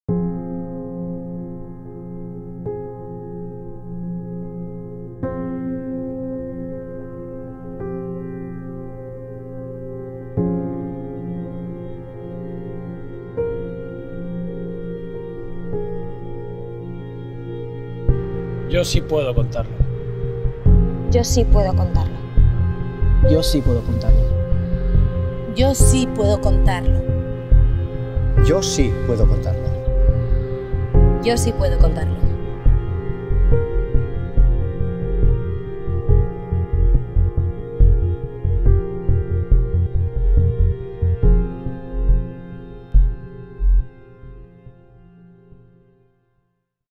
YoSiPuedoContarlo Spot campaña contra maltrato y abandono de animales de Guardia Civil
EnVerde.Net SPOT CAMPAÑA de concienciación social: La Guardia Civil presenta la campaña contra el maltrato y abandono de animales domésticos
El objetivo de la campaña, presentada hoy, es concienciar a los ciudadanos para que promuevan las medidas necesarias en la protección, ordenación y bienestar para los animales de compañía y denuncien las conductas contrarias al bienestar de los animales domésticos de las que tengan conocimiento.
La campaña YoSiPuedoContarlo se va a divulgar a través de los canales oficiales de la Guardia Civil en las diferentes redes sociales, así como en los medios de comunicación social que deseen sumarse a la iniciativa o hacerse eco de la misma.
Esta campaña se ha organizado con la inestimable y desinteresada colaboración de Backstage Producciones y de actores y actrices.

-Fuente: nota de prensa e imágenes de la Guardia Civil.-